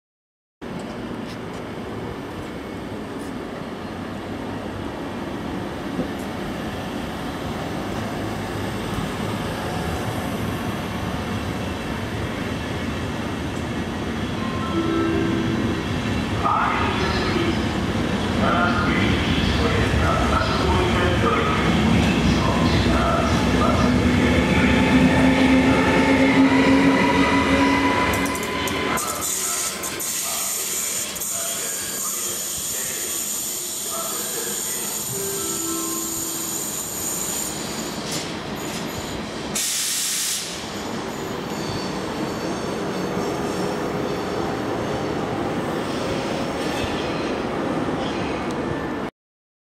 1216 233 im Bahnhof Usti nad Labem hl.n.
1216 233 verlässt den Bahnhof Usti nad Labem hl.n. in Richtung Praha